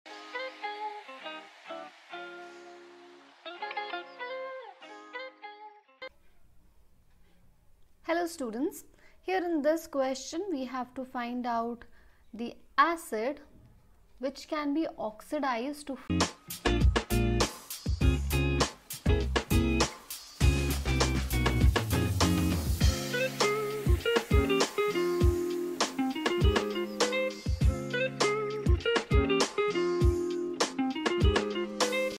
Which of the following acids can be oxidized to form a stronger acid? Select one: HPO4 HNO3 HCO2 H3…
Which of the following acids can be oxidized to form a stronger acid? Select one: HPO4 HNO3 HCO2 H3BO3 HSO2